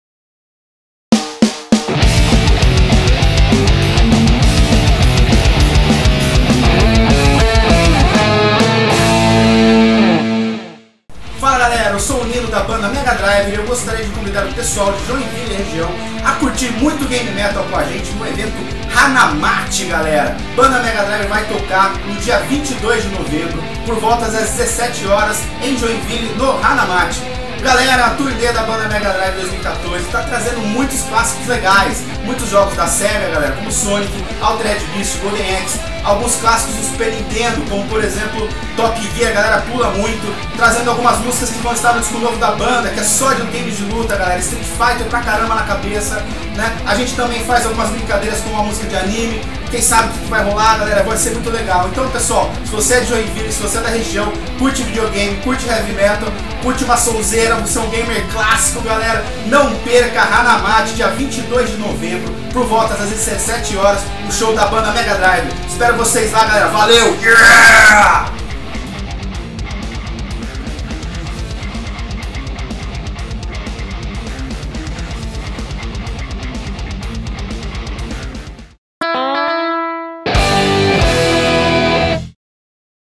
Convite: MegaDriver em Joinville no Hanamachi 2014
MegaDriver em Joinville, Sábado 22 de Novembro de 2014, no evento Hanamachi.